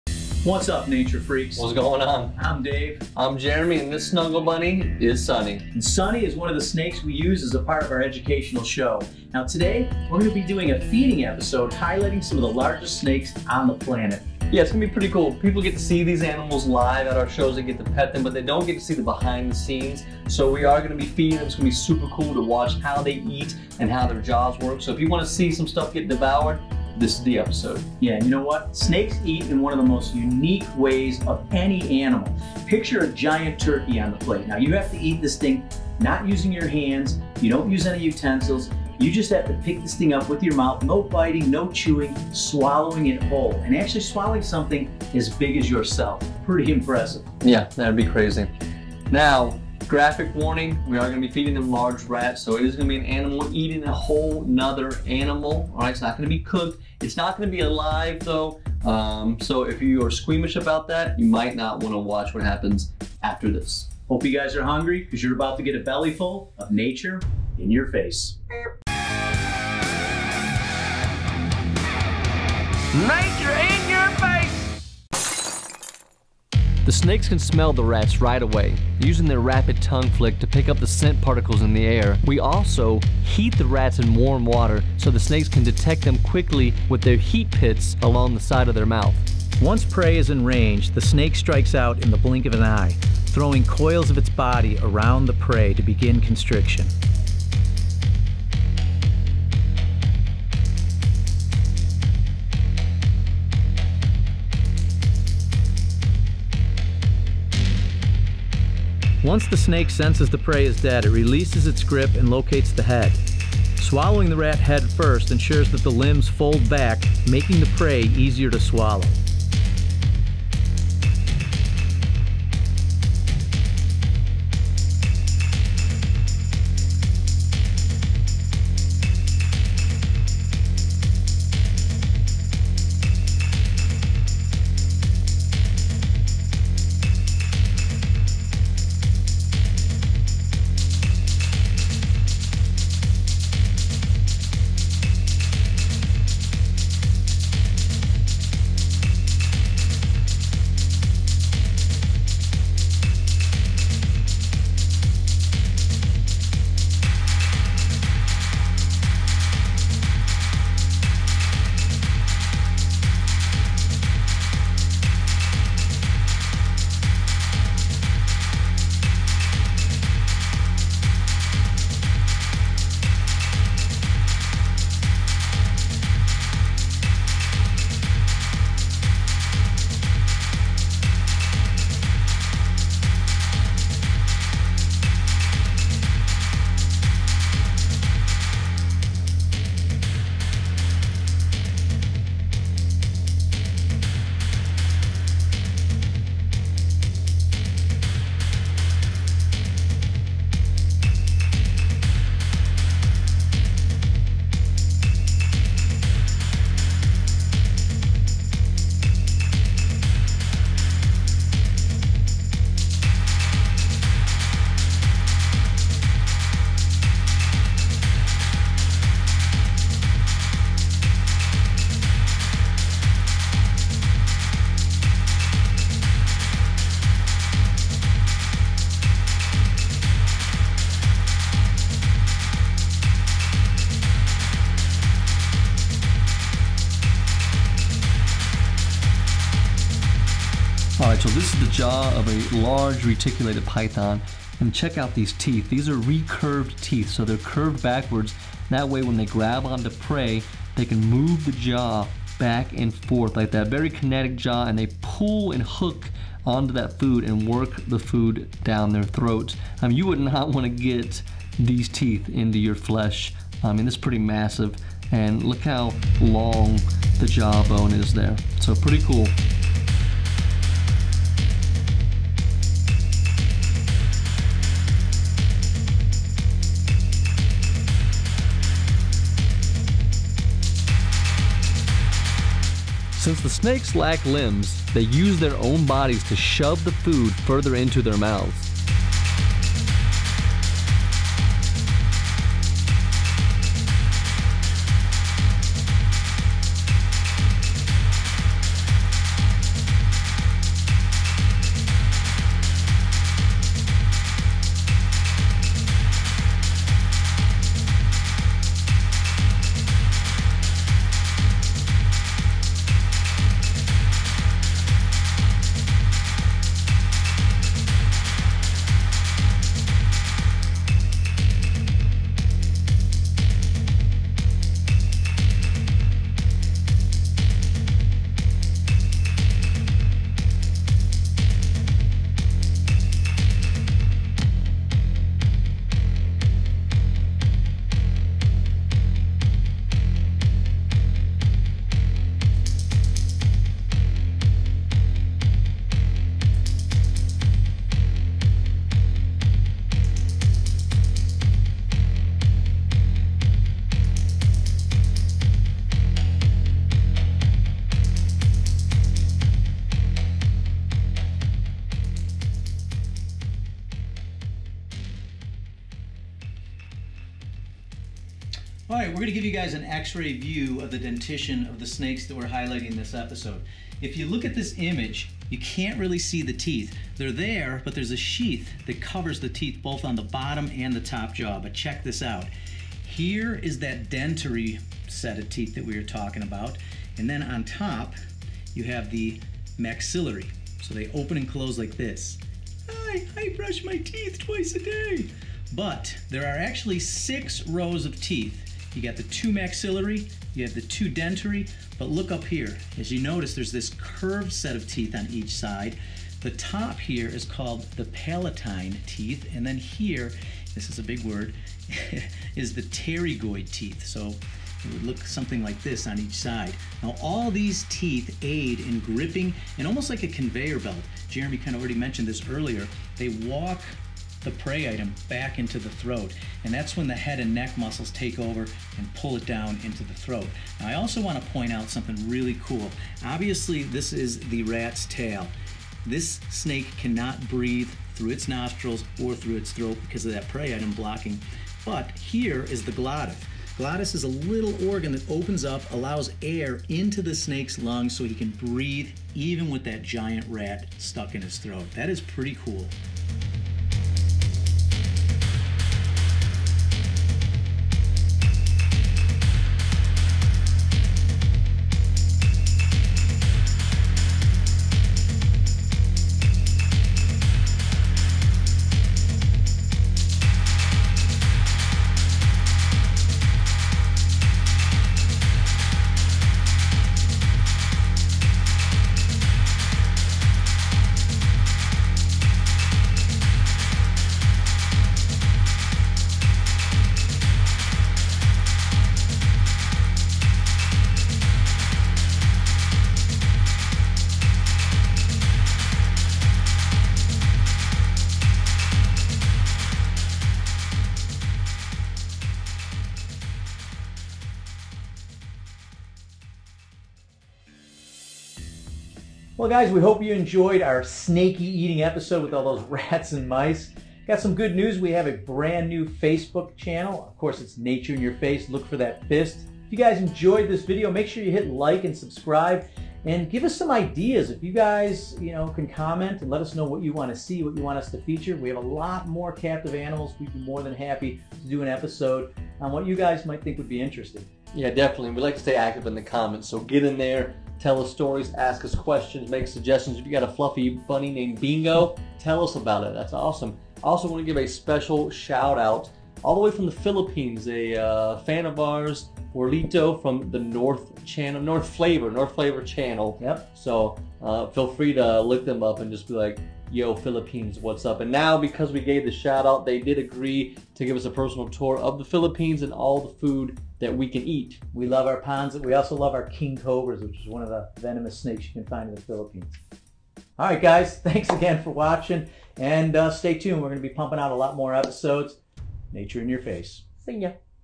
Feeding our pet snakes! Green Anaconda, Burmese Python, Reticulated Python and more.
Join us as we feed some of our large pet snakes. We promise this won't be your typical feeding video. Enjoy!    WARNING: Feeding of rats to snakes in this video, no live feeding though.